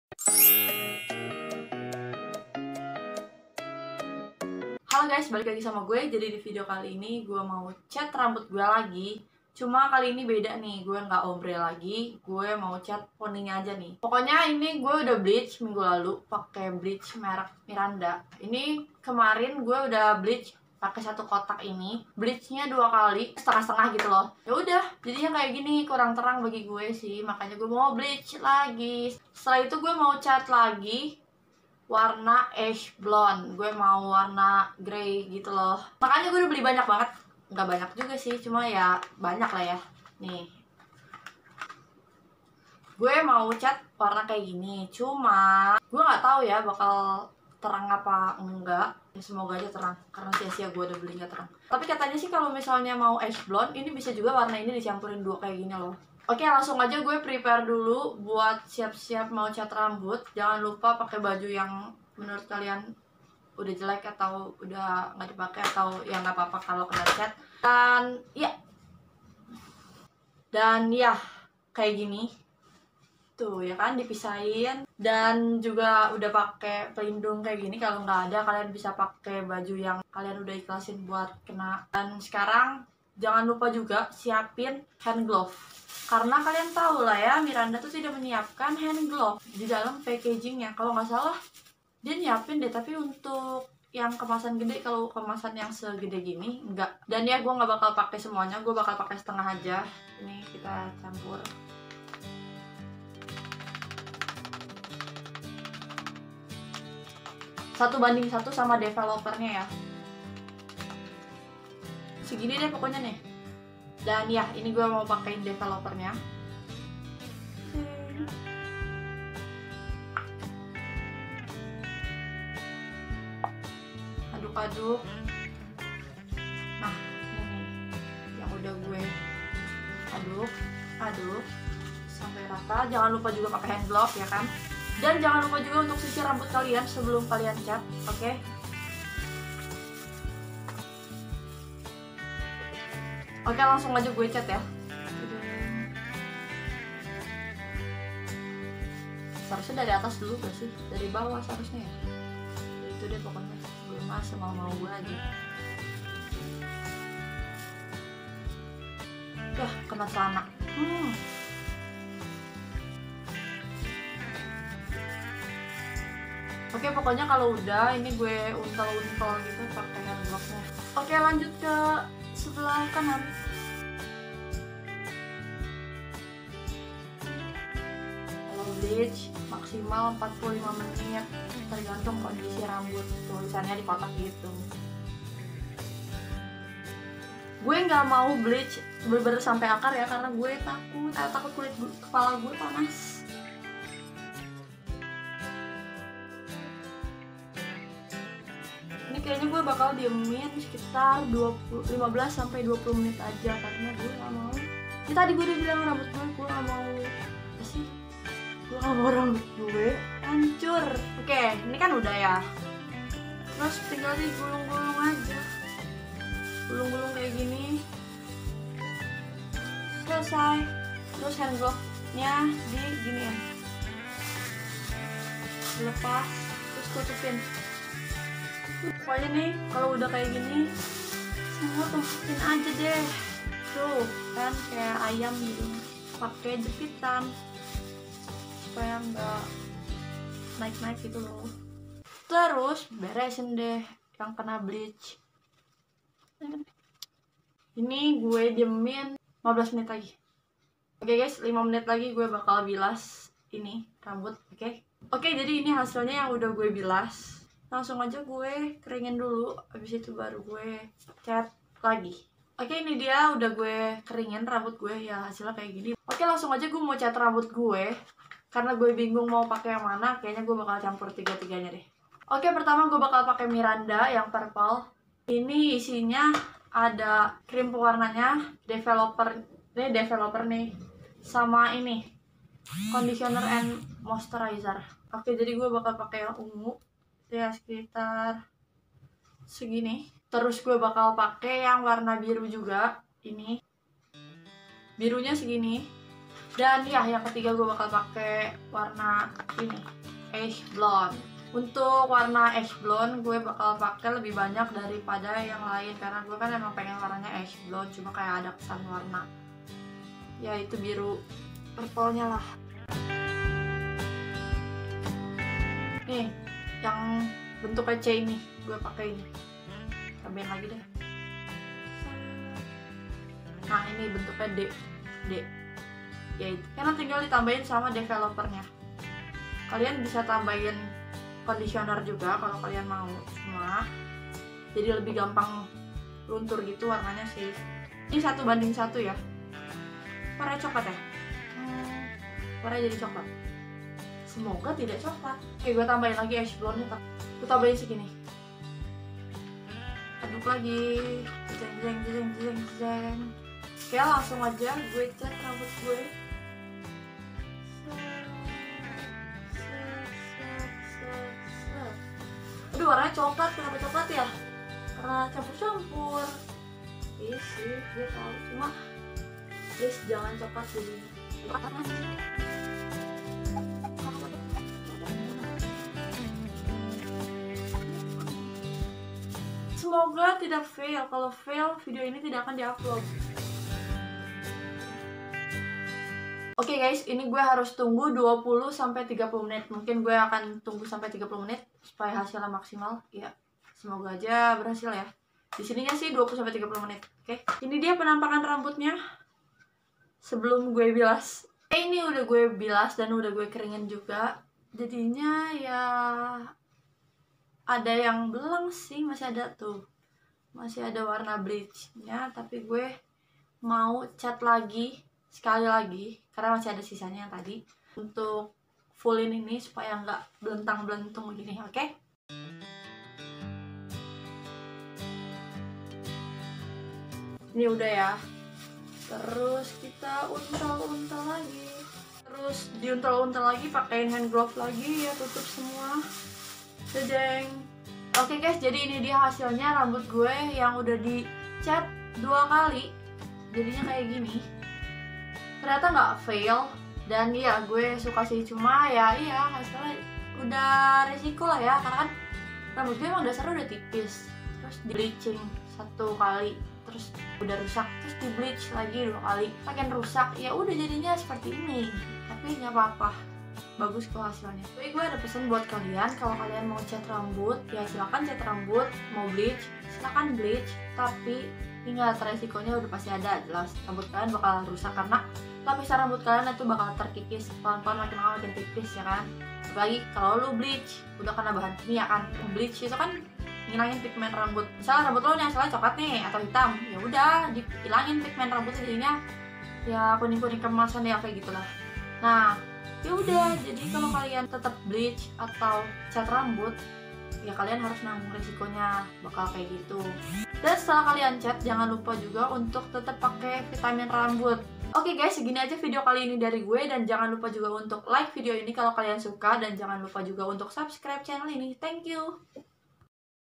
[Tutorial] Cat Rambut Ala Tiktok Pakai Miranda
Haloooo..
Kali ini gue balik lagi dengan Video Tutorial Cat Rambut yang lagi hits di Tiktok. Untuk cat rambutnya, gue pakai merk MIRANDA. Kalian bisa beli di Minimarket,Supermarket, atau E-Commerce. Untuk harganya kisaran Rp 8.500-Rp12.000-an yaa

Sebelum rambut kalian di cat, pastiin kalian harus bleaching dulu yaa!
kenapa harus bleaching? Supaya warna catnya lebih keliatan (lebih nyala).
Kalo gak bleaching bisa gak kak? Tentu bisa, tapi hasilnya jelas berbeda dengan rambut yang sudah di bleaching.

Gue ga tau kenapa commentnya di off-in sama YT 😭